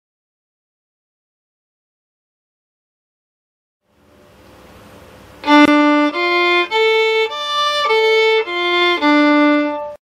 ABRSM Violin Exam Grade 2 scales and arpeggios - D minor arpeggio (separate bow)
ABRSM Violin Exam Grade 2 scales and arpeggios
D minor arpeggio (separate bow)
Played by Mike Chau